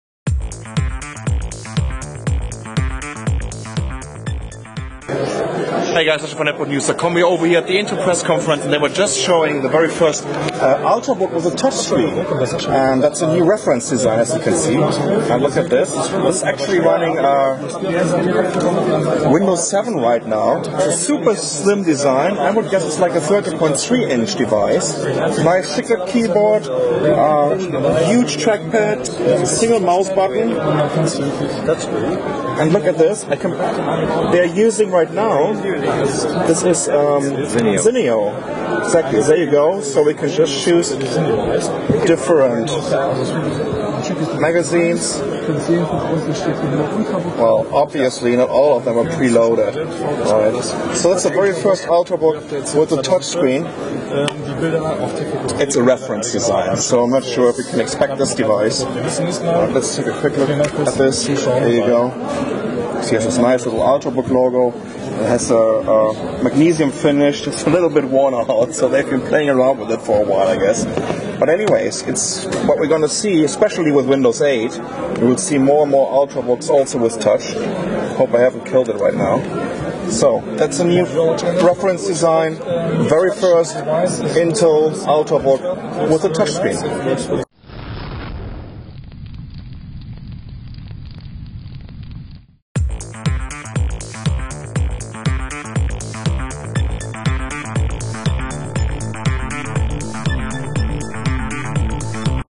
Touchscreen Ultrabook Reference Design Hands on at CeBit 2012
touch screen ultrabook Intel has been talking about their next generation of Ultrabooks and touch screens have been a big part of that world.  Today at their press event at CeBit they showed off a reference design for a touch screen Ultrabook and as you can tell from the video below it looks gorgeous.  They showed off Zinio which is an an app for reading magazines, we were easily able to flip through pages as well as interact with the page content.